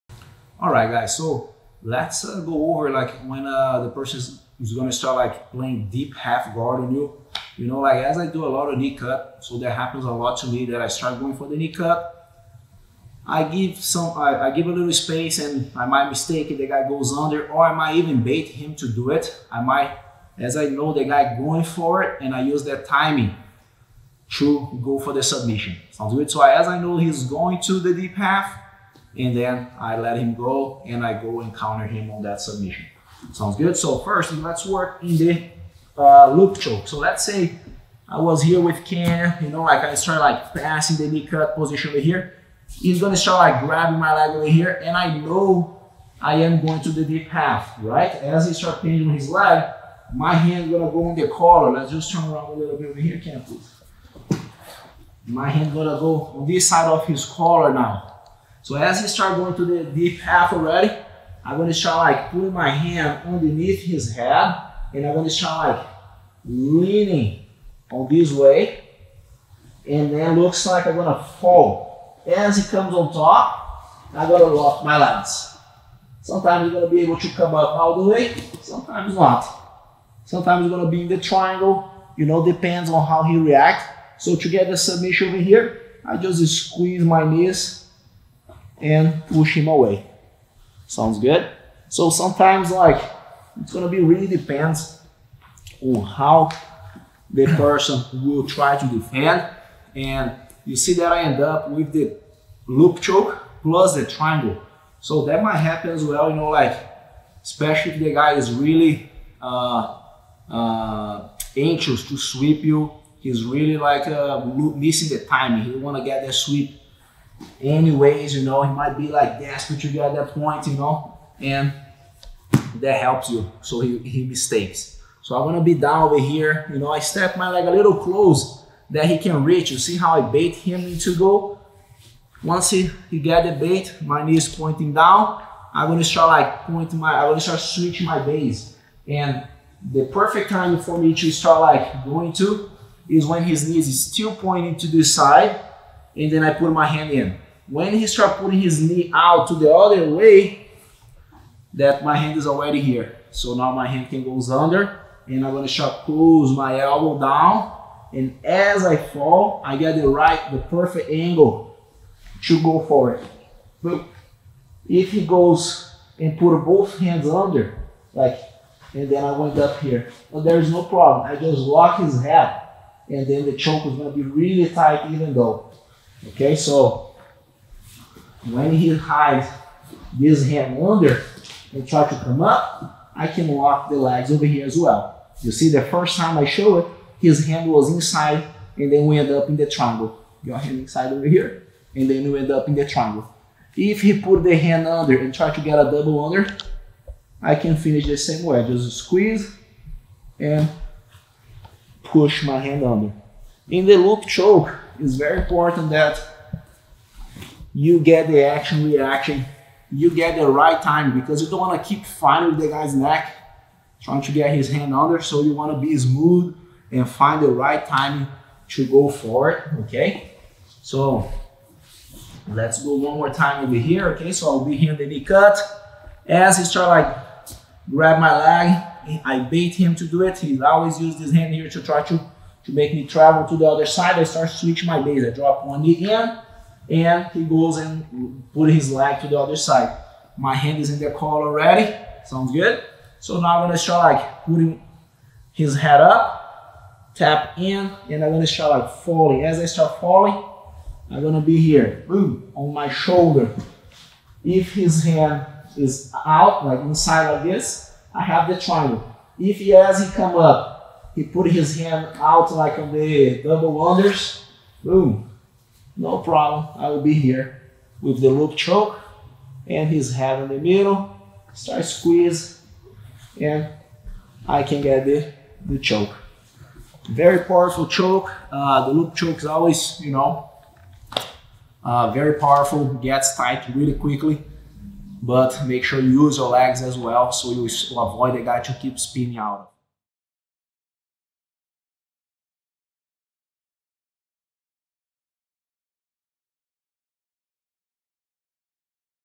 Deep Half Guard Counter With Loop Choke by Frederico Silva
DEEP HALF GUARD COUNTER WITH LOOP CHOKE

Frederico Silva teaches the Deep Half Guard Counter With Loop Choke in this BJJ Training video.

Frederico Silva is an accomplished BJJ black belt competitor and instructor representing Lucas Lepri BJJ.    This is an excerpt from COUNTERS FROM EVERYWHERE available from BJJFanatics.com.